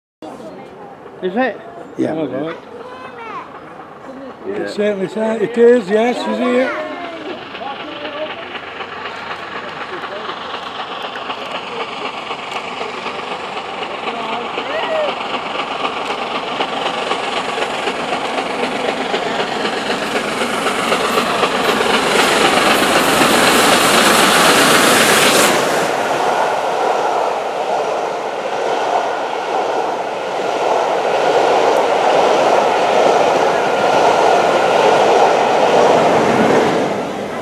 Flying Scotsman 60103 29/06/19
Flying Scotsman heads North past Birchwood Lane with the Yorkshireman railtour.